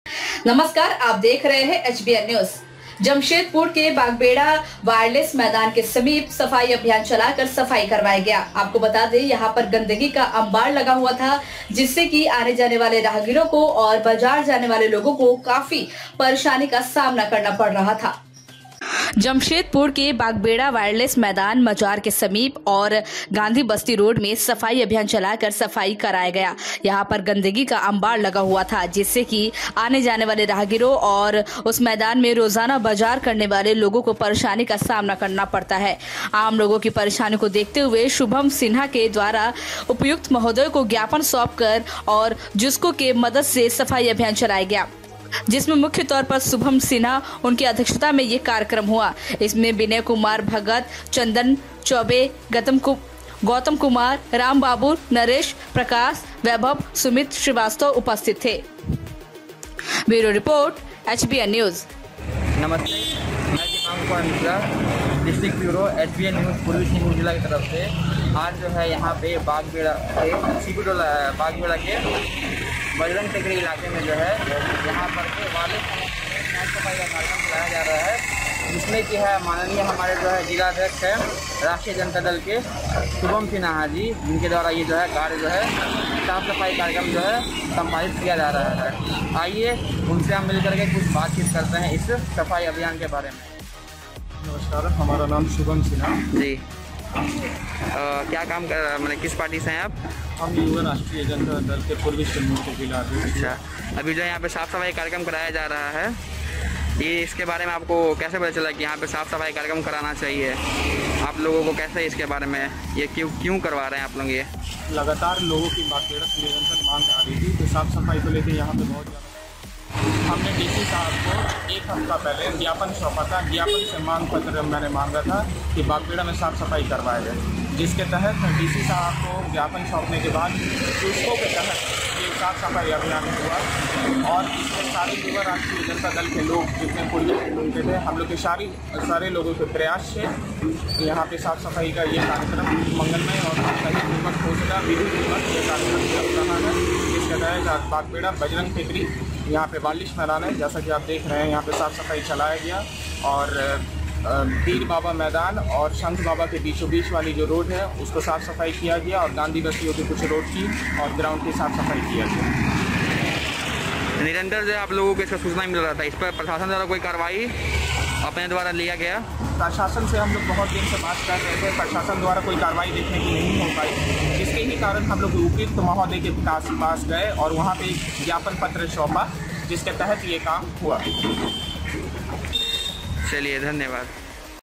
सफ़ाई अभियान चलाकर सफ़ाई करवाई, गया, देखें | HBN News
जमशेदपुर के बागबेड़ा वायरलेस मैदान, मज़ार के समीप और गांधी बस्ती रोड पर सफ़ाई अभियान चलाकर सफ़ाई करवाई, गया, देखें | HBN News | Jo Sach vhi Khabar